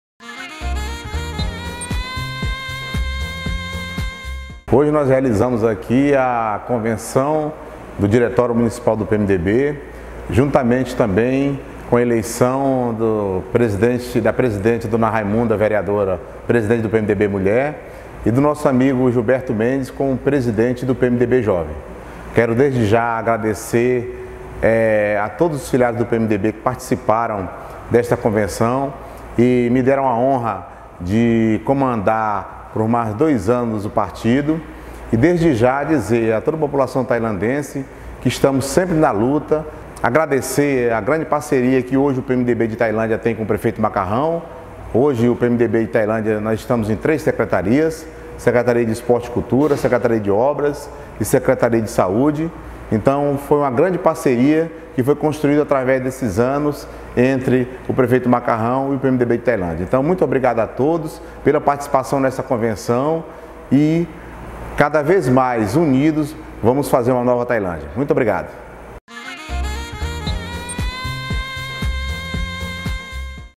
Convenção do diretório que houve Terça-Feira na Câmara Municipal.
Melqui Gonçalves Presidente do PMDB, fala da Convenção do diretório que houve Terça-Feira na Câmara Municipal.